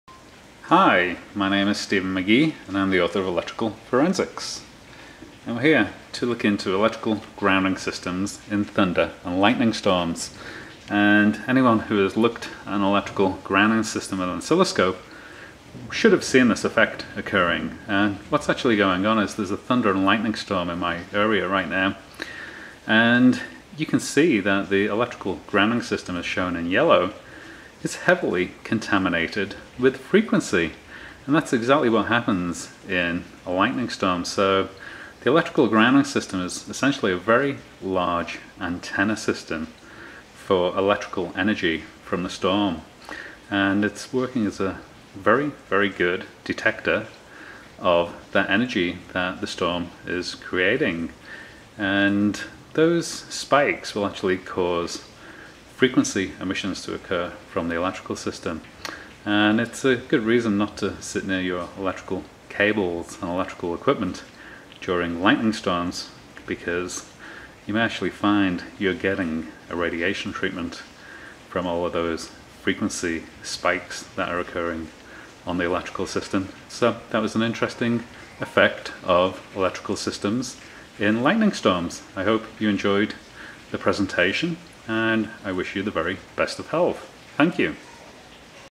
Electrical Earthing Systems During Lightning Storms
Electrical Earthing Systems During Lightning Storms.   Red is 120 volt AC and yellow is electrical ground connection. Industry standard for utility action is 0.5 volt RMS due to the known biohazard that it presents I can recommend the books Dirty Electricity: Electrification and the Diseases of Civilization by MD Samuel Milham and Electrocution of America: Is Your Utility Company Out to Kill You? by Russ Allen